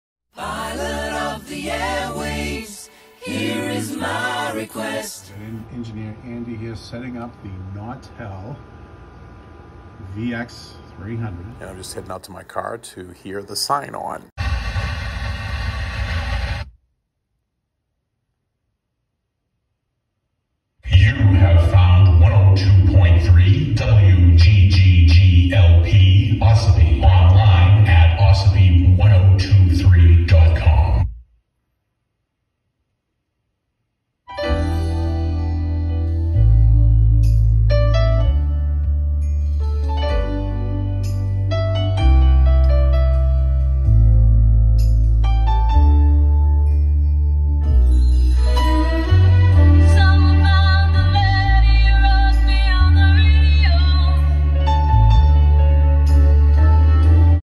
WGGG-LP is on-air
We launched WGGG-LP tonight, 102.3 on the FM dial all around Ossipee Lake in NH.  Ongoing technical improvements continue to be made.